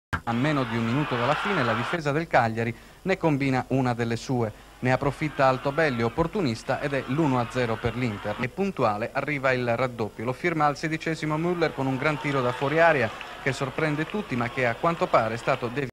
1982/83, Serie A, Cagliari - Inter 0-2 (03)
Nel Campionato di Serie A del 1982/83 nella terza giornata del girone di Andata, Cagliari - Inter finisce 0-2.